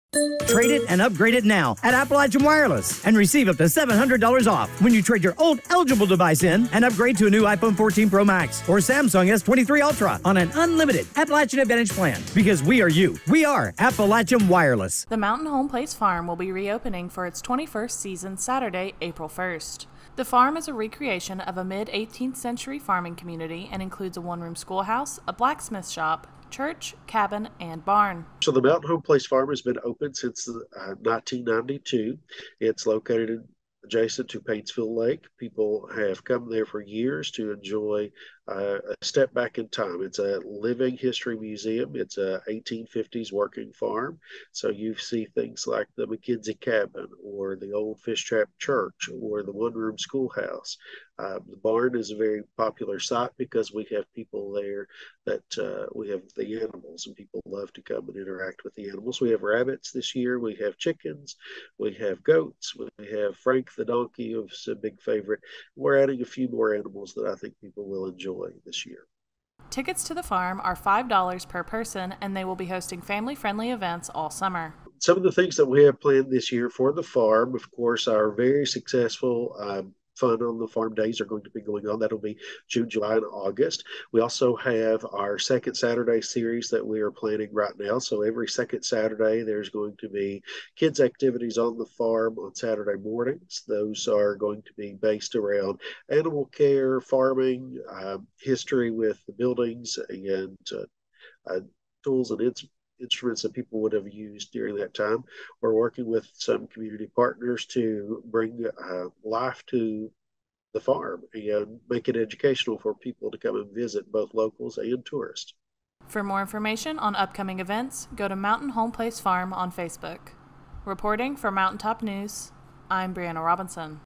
Mountain Homeplace Farm Reopens April 1
The Mountain Homeplace Farm will reopen for its twenty-first season on Saturday, April 1.